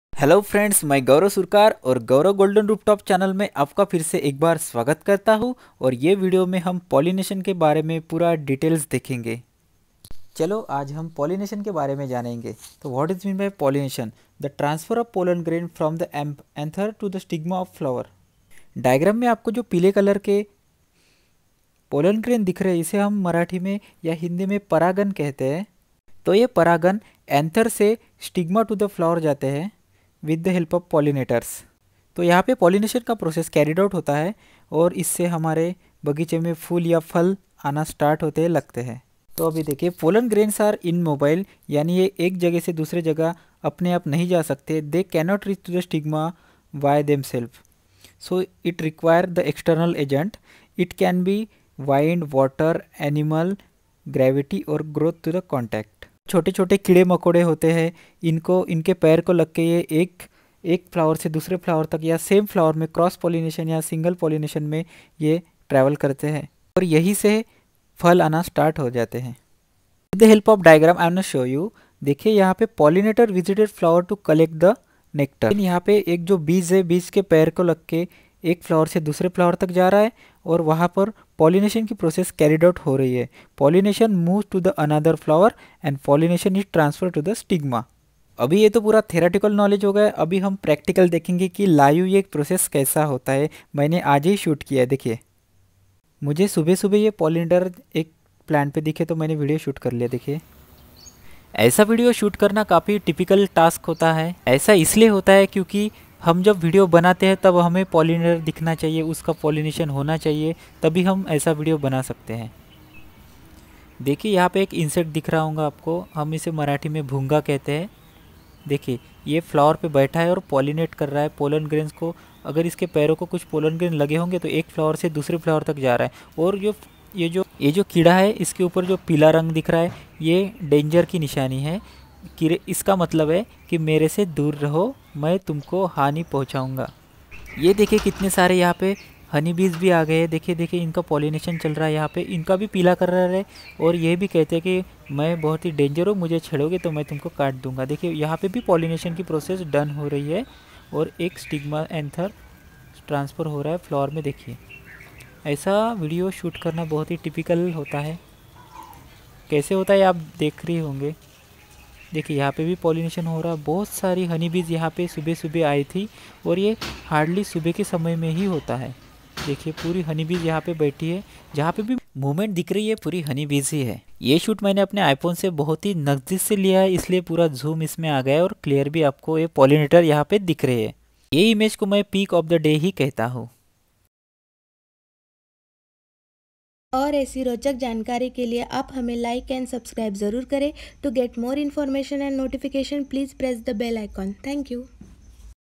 Pollination | Pollinating Agents In Plant | Live Pollination In Hindi
Pollination |Pollinating Agents In Plant | Live Pollination In Hindi In this video im going to tell about pollination.

Urban farming
Cauliflower plant
Edible garden